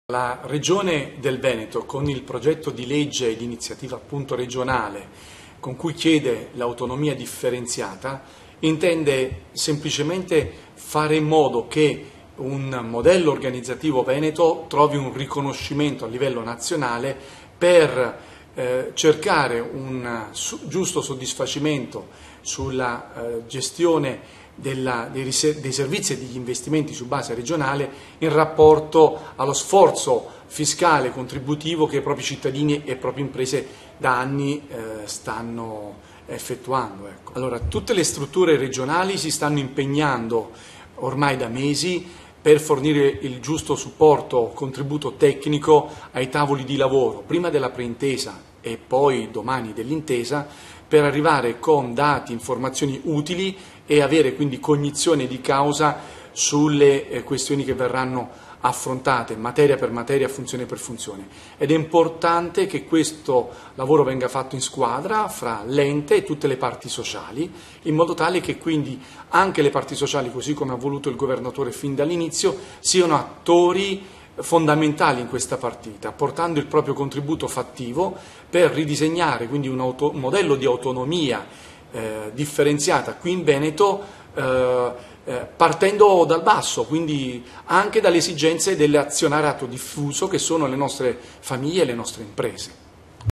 Intervista a Gianluigi Masullo, Regione Veneto
4 maggio 2018 - Mogliano Veneto. Incontro di studio di Arsenale 2022 sull'art.4 dell'Accordo preliminare in merito all'intesa con il Governo per l'autonomia del Veneto. Intervista a Gianluigi Masullo, Regione Veneto